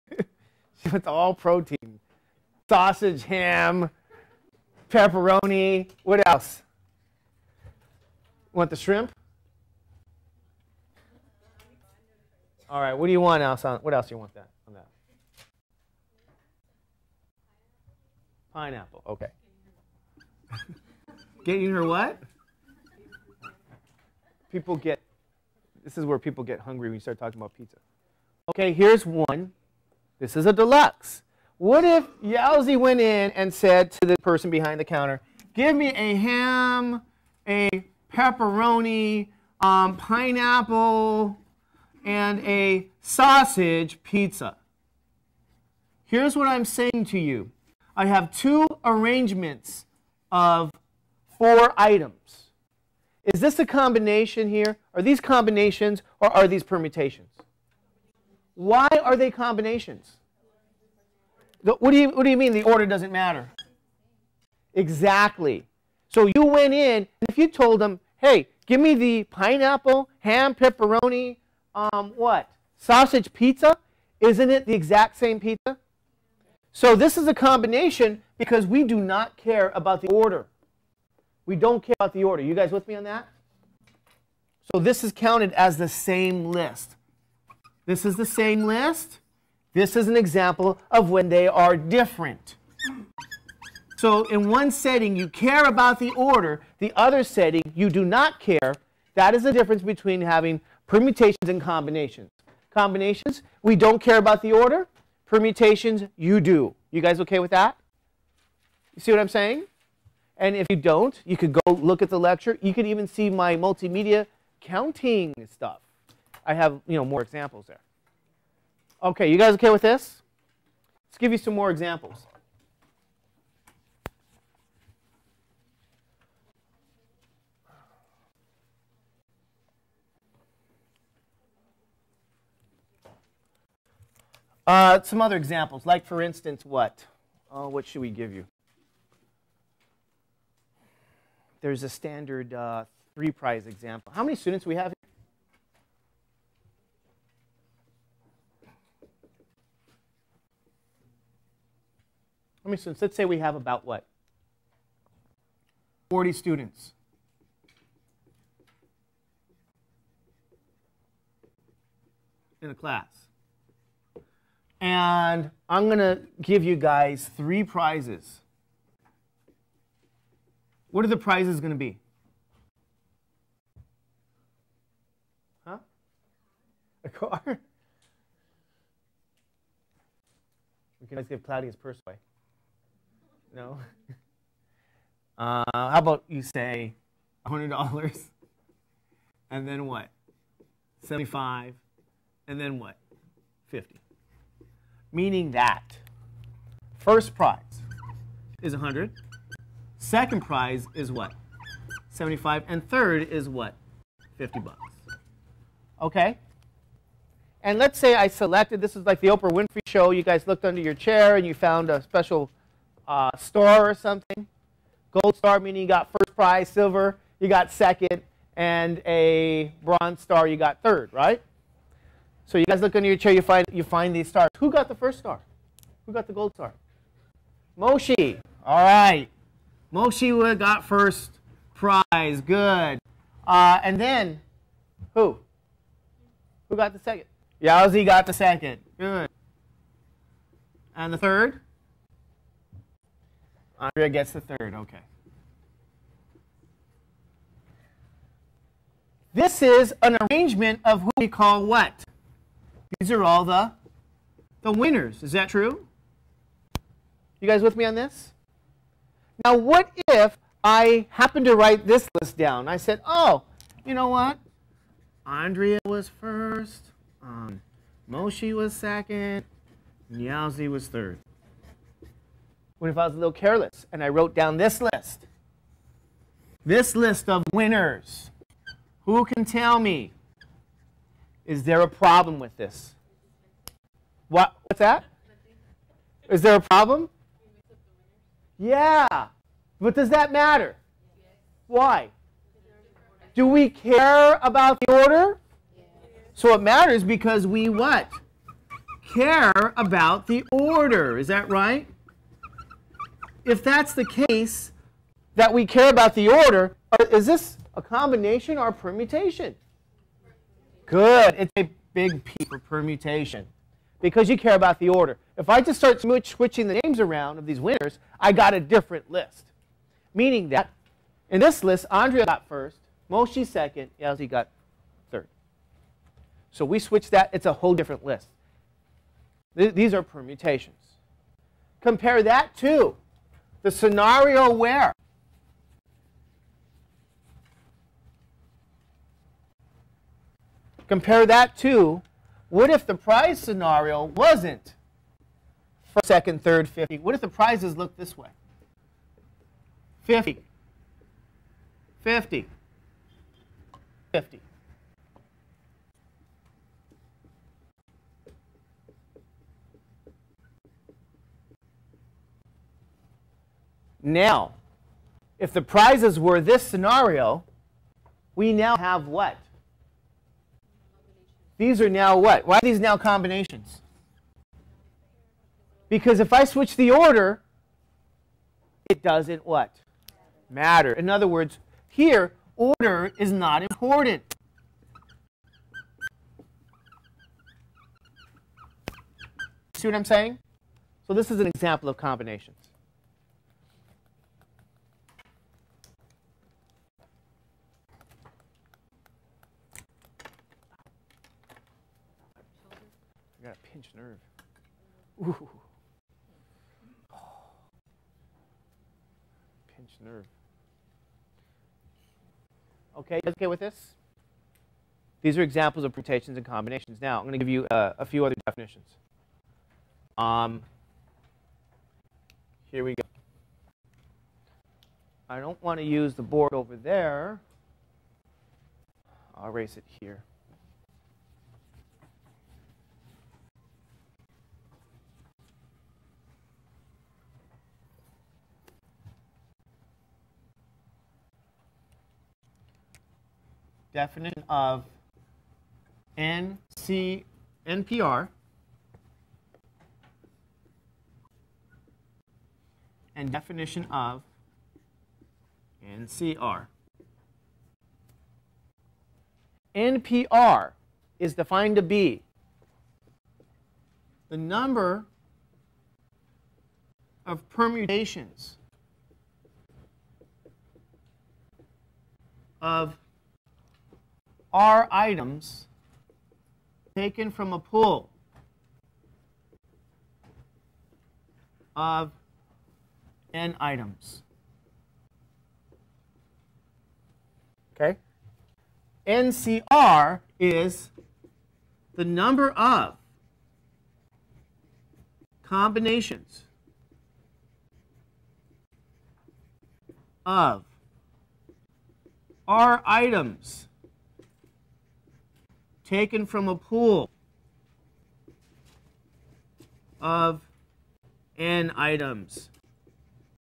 Daniel Judge- Statistics Lecture 8 (part 5)
This lecture covers counting and probability. Daniel Judge at East Los Angeles College, Spring 2008.